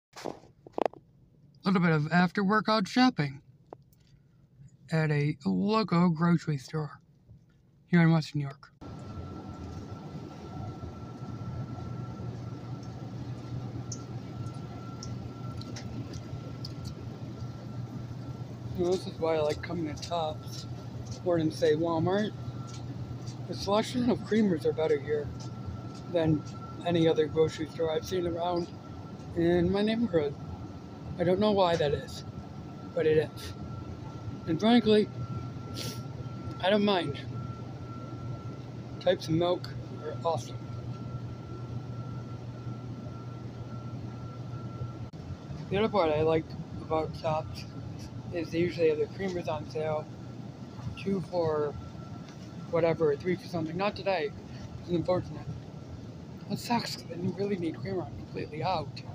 shopping food foodie creamer tops topsmarket davidslife walmart foodvlog foodlover wny
Glad everyone likes the creamer section at my local Tops Market! Look for more isles to come soon!